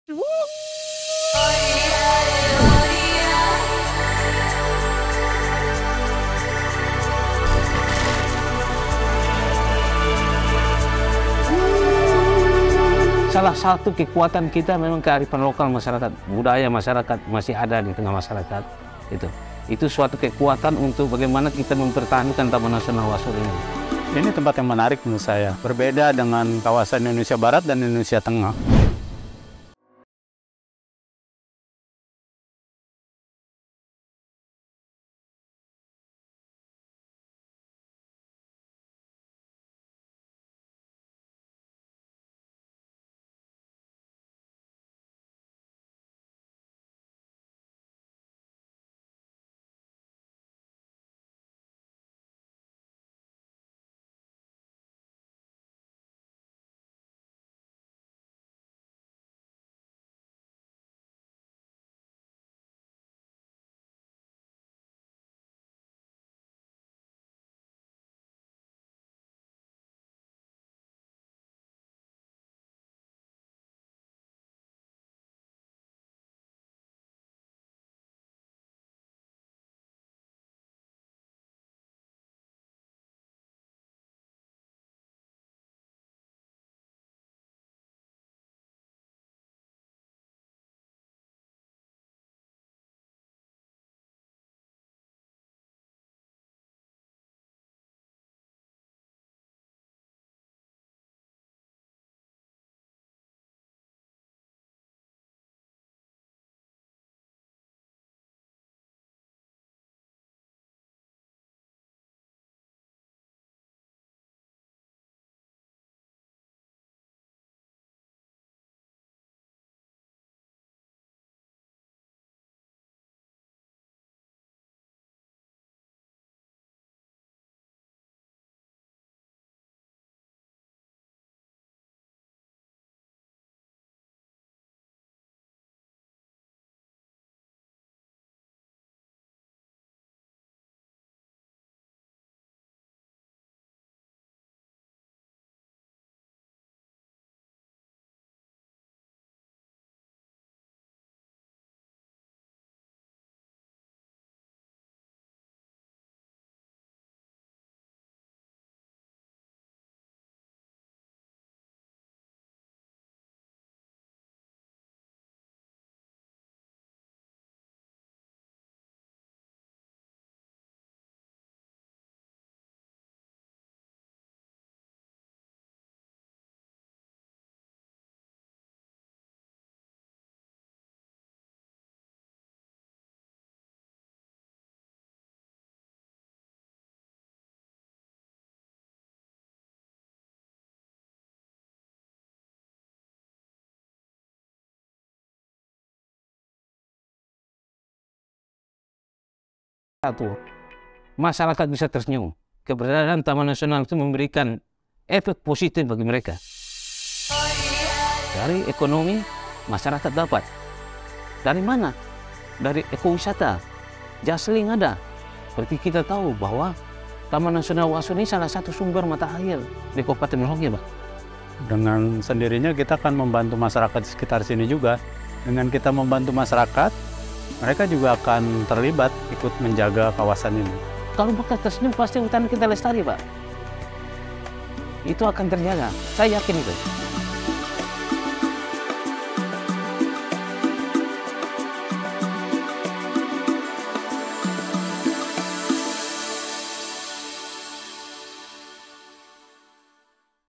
Serengeti Papua di Taman Nasional Wasur | Lahan Basah Terbesar Di Papua.
Pada video ini kami menyuguhkan potensi luar biasa dariTaman Nasional Wasur Merauke Papua Selatan. Sebagai kawasan wetland (lahan basah) yang luas dengan beragam keanekaragaman hayati yang khas.
Videografer :
Ricky Martin @ricky_jalanjalan
Aga Akbel Pratama
Firly
*Terima kasih kepada :
Hesco La Hisa, Penyuluh Taman Nasional Wasur
Riza Marlon, Wildlife Photographer

Video editor :
Ricky Martin
Aga Akbel